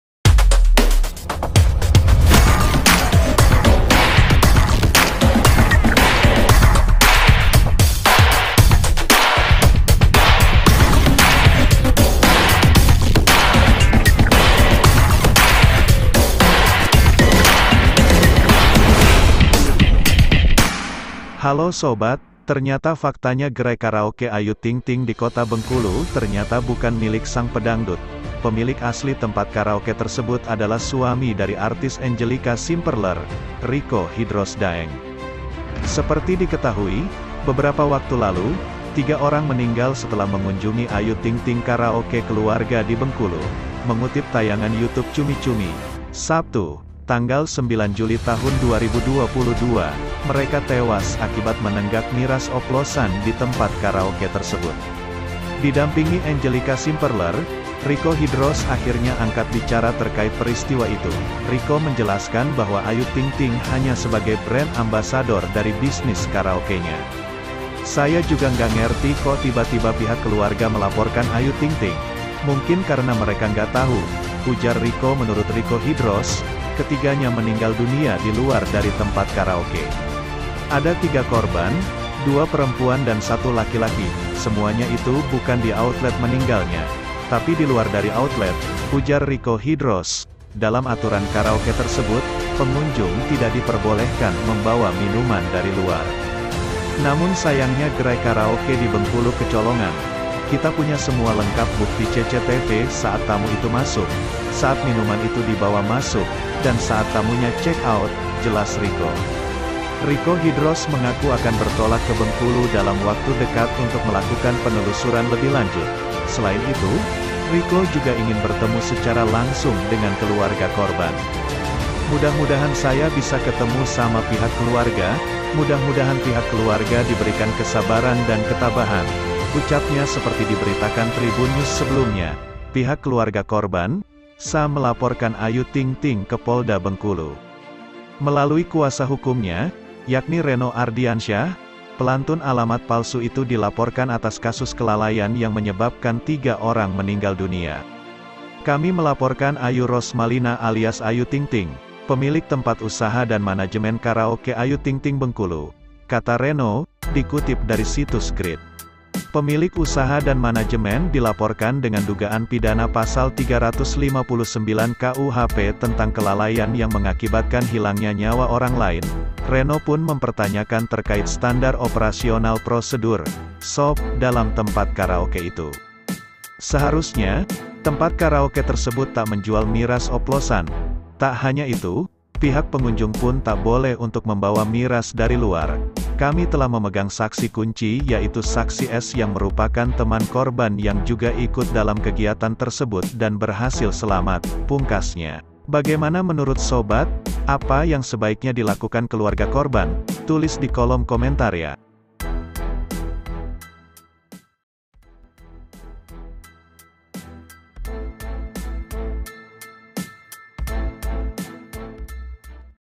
Ayu ting ting dilaporkan ke polisi akibat tewasnya orang di tempat karaoke ting ting Kasus ting ting
Ayu ting ting dilaporkan ke polisi oleh keluarga korban karena diduga pemilik tempat ting ting karaoke,
higlight :
Kasus ting tingayu ting ting cemburu mantanmu karaoke karaoke acoustic ayu ting ting karaoke karaoke ting ting
lagu karaoke